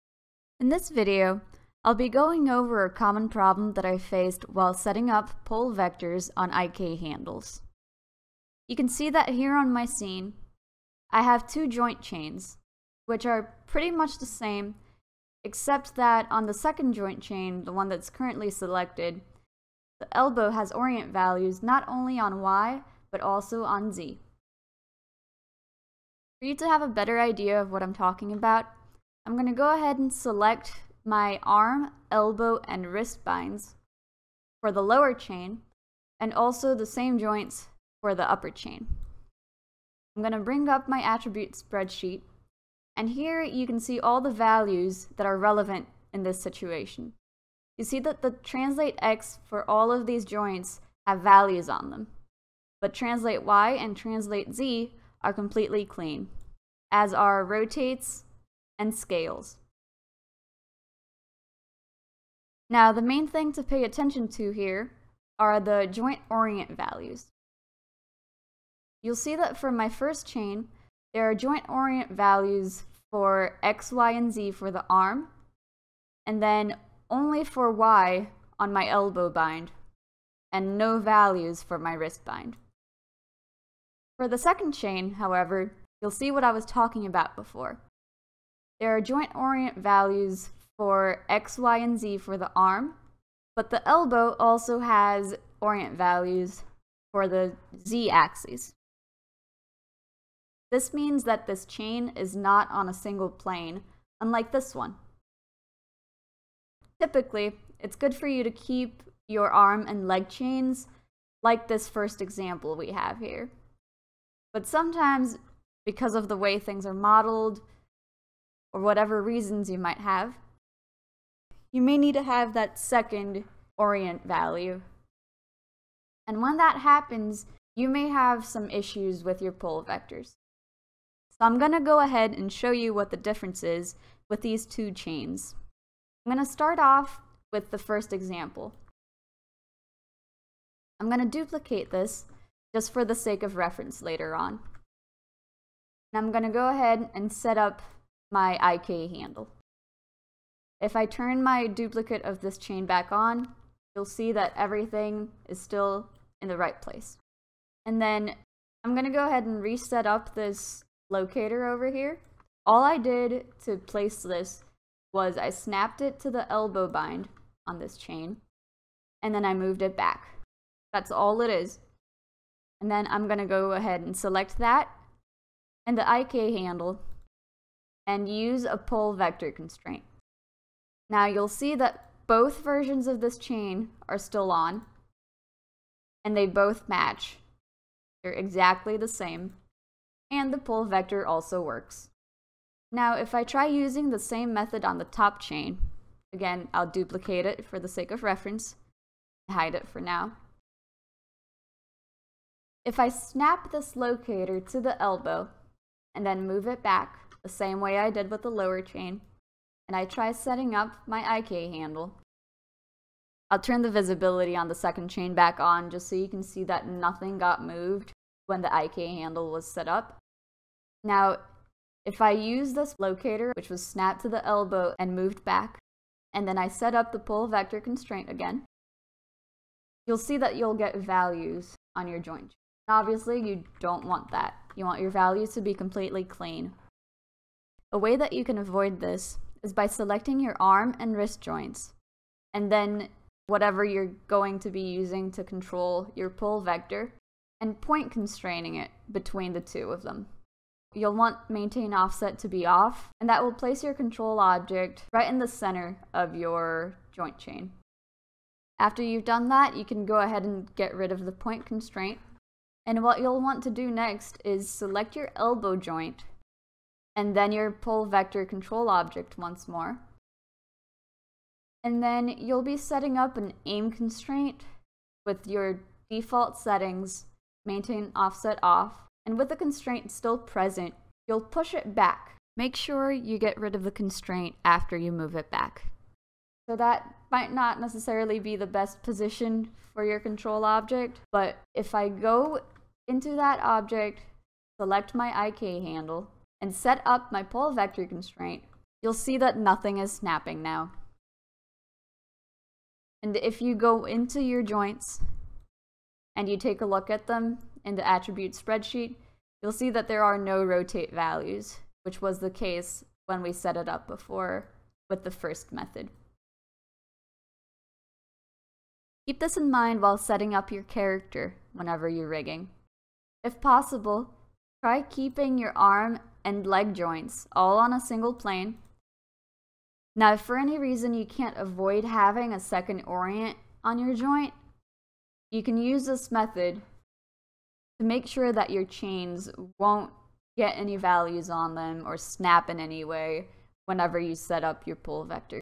005 - Pole Vectors
Having problems with IK joints snapping out of position when setting up a pole vector constraint? This video covers how to set up an ideal joint chain for IK handles, and also how to make sure you can avoid rotations on a chain that is not on a single plane.